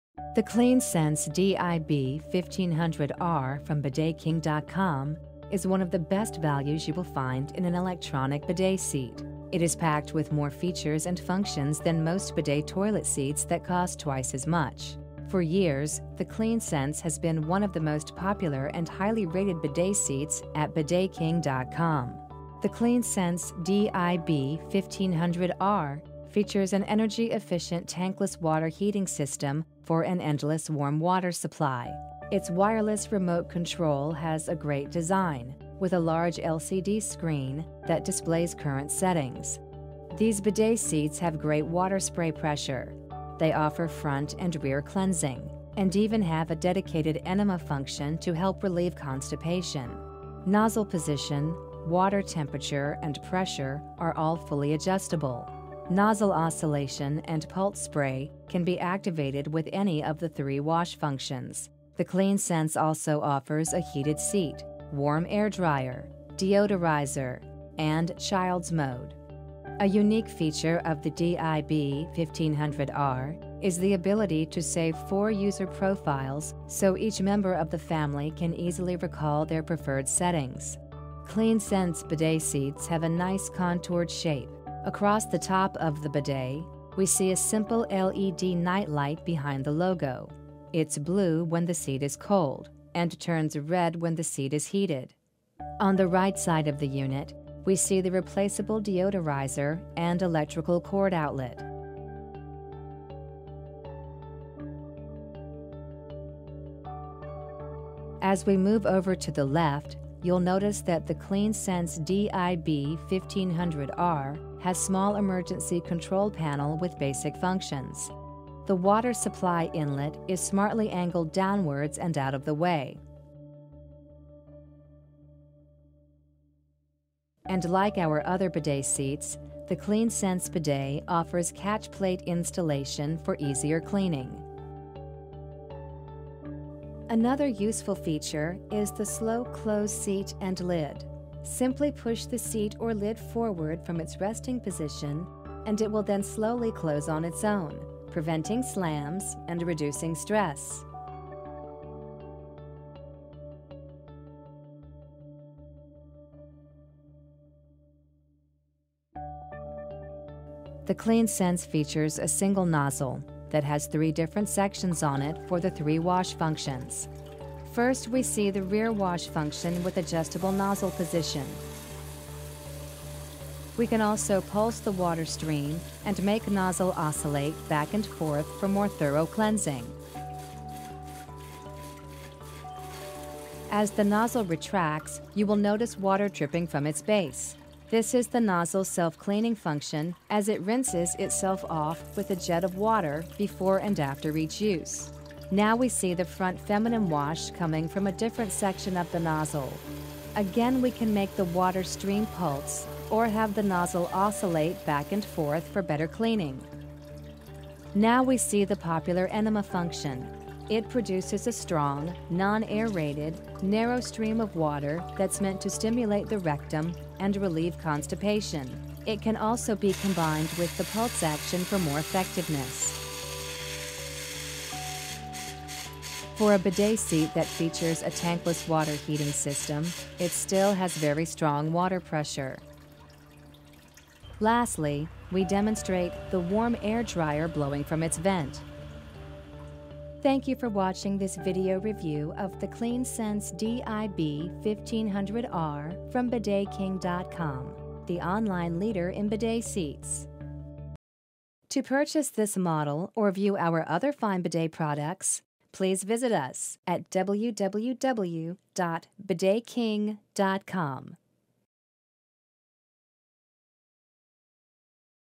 Clean Sense dib-1500R Bidet Review - BidetKing.com
This video is a review of the Clean Sense dib-1500R bidet toilet seat from BidetKing.com.

The Clean Sense dib-1500R from BidetKing.com is of the best values you will find in an electronic bidet seat.  It is packed with more features and functions than most bidet toilet seats that cost twice as much.  For years, the Clean Sense has been one of the most popular and highly rated bidet seats at BidetKing.com.
The Clean Sense dib-1500R features an energy efficient tankless water heating system for an endless warm water supply.  Its wireless remote control has a great design with a large LCD screen that displays current settings.

These bidet seats have great water spray pressure.  They offer front and rear cleansing, and even have a dedicated enema function to help relieve constipation.  Nozzle position, water temperature and pressure are all fully adjustable.  Nozzle oscillation and pulse spray can be activated with any of the 3 wash functions. The Clean Sense also offers a heated seat, warm air dryer, deodorizer, and child's mode.

A unique feature of the dib-1500R is the ability to save 4 user profiles so each member of the family can easily recall their preferred settings.

Clean Sense bidet seats have a nice contoured shape. Across the top of the bidet, we see a simple LED night light behind the logo. It's blue when the seat is cold, and turns red when the seat is heated. On the right side of the unit, we see the replaceable deodorizer and electrical cord outlet. As we move over to the left, you'll notice that the Clean Sense dib-1500R has small emergency control panel with basic functions.

The water supply inlet is smartly angled downwards and out of the way.  And like our other bidet seats, the Clean Sense bidet offers catch plate installation for easier cleaning.
Another useful feature is the slow close seat and lid.  Simply push the seat or lid forward from its resting position and it will then slowly close on its own.

The Clean Sense features a single nozzle that has 3 different sections on it for the 3 wash functions. First, we see the rear wash function with adjustable nozzle position. We can also pulse the water stream and make nozzle oscillate back and forth for more thorough cleansing. As the nozzle retracts, you will notice water dripping from its base.  This is the nozzle's self cleaning function as it rinses itself of with a jet of water before and after each use

Now, we see the front feminine wash coming from a different section of the nozzle. Again we can make the water stream pulse or have the nozzle oscillate back and forth for better cleaning.

Now, we see the popular enema function.  It produces a strong, non aerated, narrow stream of water that's meant to stimulate the rectum and relieve constipation.  It can also be combined with the pulse action for more effectiveness. For a bidet seat that features a tankless water heating system, it still has very strong  water pressure.

Lastly, we demonstrate the warm air dryer blowing from its vent.